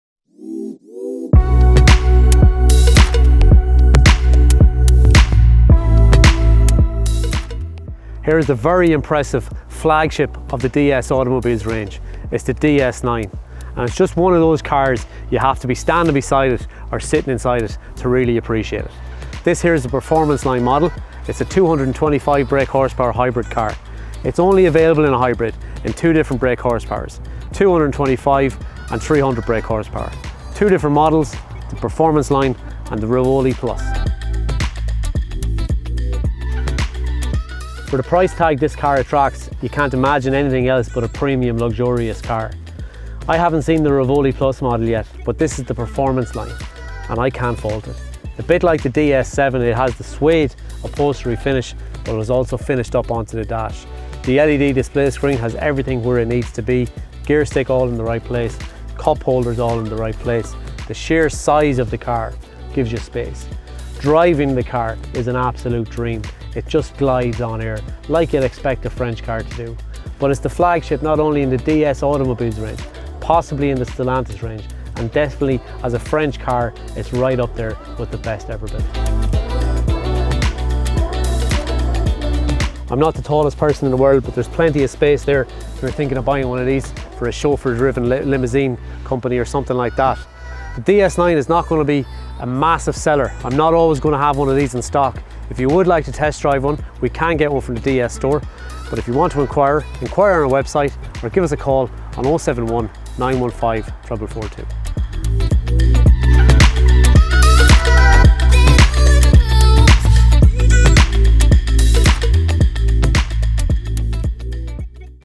DS Automobiles DS9 Short Review at Kevin Egan Cars
We recently did a short review of the DS Automobiles DS9 at the @sligoraces1262 , a luxurious car just had to be brought to a session like this to show it off, The DS9 is available in Hybrid technology and two different trim levels 225BHP & 300BHP @PeugeotIreland1